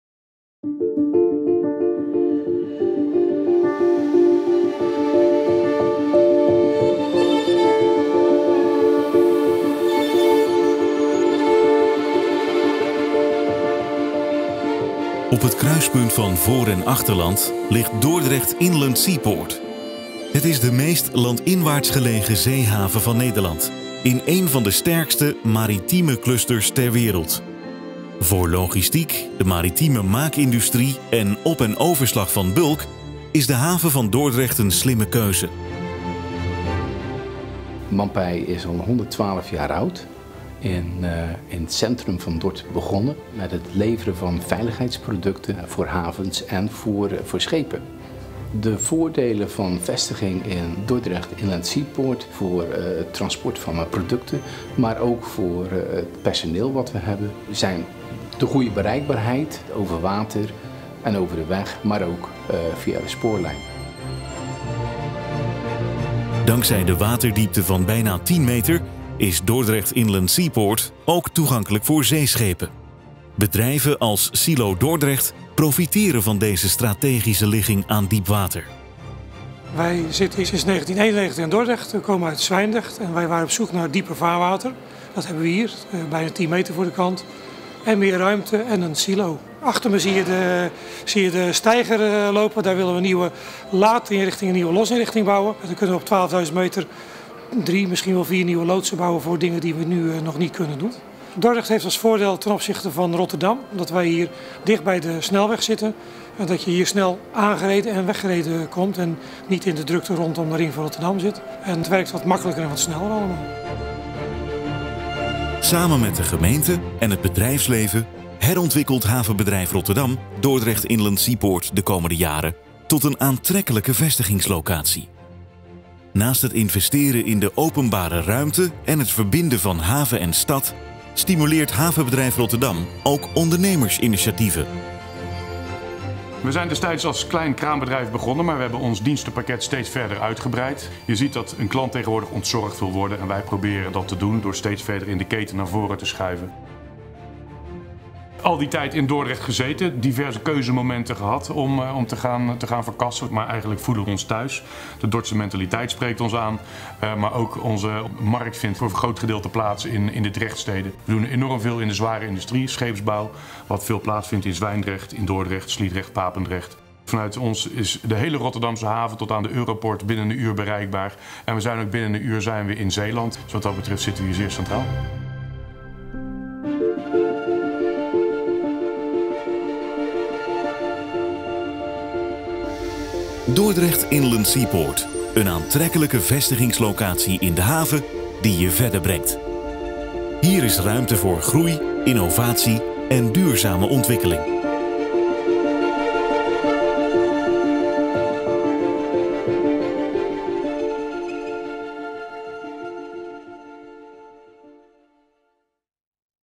Dordrecht Inland Seaport. Gets you closer.
Dordrecht Inland Seaport is de meest landinwaarts gelegen zeehaven van Nederland en is qua ligging en bereikbaarheid uniek. Het havengebied ligt op het kruispunt van voor- en achterland en dankzij de waterdiepte van 10 meter is de haven ook toegankelijk voor zeeschepen. De haven van Dordrecht is zeer aantrekkelijk voor de maritieme maakindustrie, logistiek en overslag van bulk. Bent u op zoek naar een vestigingslocatie in een van ’s werelds belangrijkste maritieme clusters, dichtbij de haven van Rotterdam?